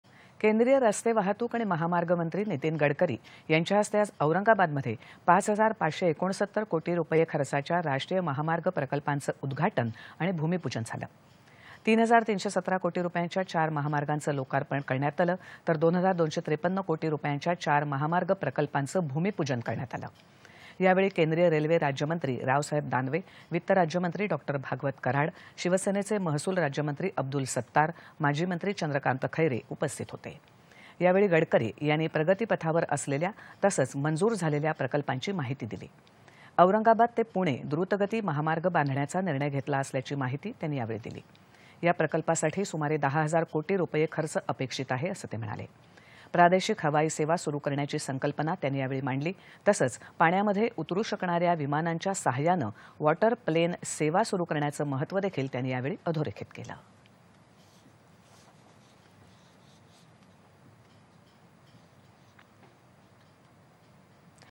नितीन गडकरी यांच्या हस्ते औरंगाबादमध्ये राष्ट्रीय महामार्ग प्रकल्पांचं उद्घाटन
दि.२४.०४.२०२२ :
केंद्रीय रस्तेवाहतूक आणि महामार्गमंत्री नितीन गडकरी यांच्या हस्ते आज औरंगाबादमध्ये पाच हजार पाचशे ६९ कोटी रुपये खर्चाच्या  राष्ट्रीय महामार्ग प्रकल्पांचं उद्घाटन आणि भूमीपूजन झालं.  3 हजार 317 कोटी रुपयांच्या चार महामार्गांचं लोकार्पण करण्यात आलं तर 2 हजार 253 कोटी रुपयांच्या चार महामार्ग प्रकल्पांचं भूमीपूजन करण्यात आलं. यावेळी केंद्रीय रेल्वे राज्यमंत्री रावसाहेब दानवे, वित्त राज्यमंत्री डॉ. भागवत कराड, शिवसेनेचे महसूल राज्यमंत्री अब्दुल सत्तार, माजी मंत्री चंद्रकांत खैरे उपस्थित होते. यावेळी गडकरी यांनी प्रगतीपथावर असलेल्या तसंच मंजूर झालेल्या प्रकल्पांची माहिती दिली. औरंगाबाद ते पुणे द्रुतगती महामार्ग बांधण्याचा निर्णय घेतला असल्याची माहिती त्यांनी यावेळी दिली. या प्रकल्पासाठी सुमारे दहा हजार कोटी रुपये खर्च अपेक्षित आहे, असं ते म्हणाले. प्रादेशिक हवाई सेवा सुरू करण्याची संकल्पना  त्यांनी यावेळी मांडली. तसंच पाण्यामध्ये उतरू शकणाऱ्या विमानांच्या सहाय्यानं वॉटर प्लेन सेवा सुरू करण्याचं महत्त्व अधोरेखित केलं.